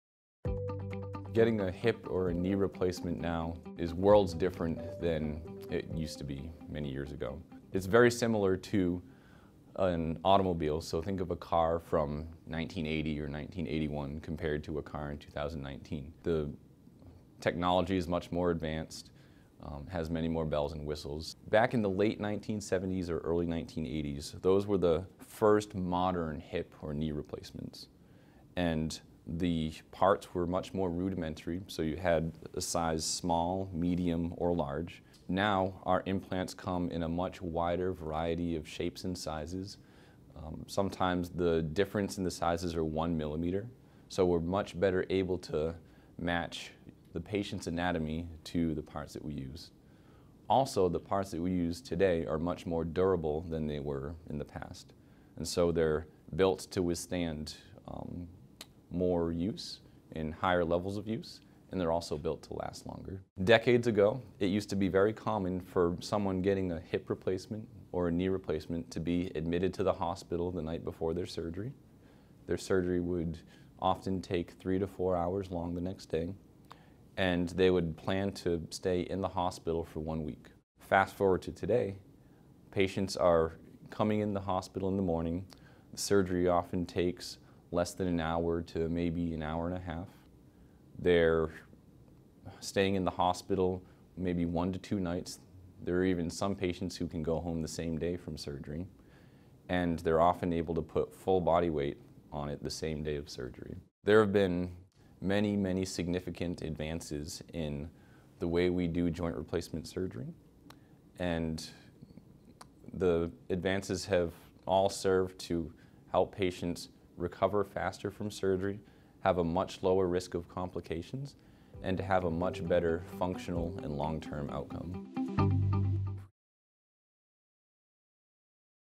Hip to Technology: Advancements in Joint Replacement Surgery
Dr. Timothy McGlaston of IHA, talks about recent advancements in hip and knee replacement surgery.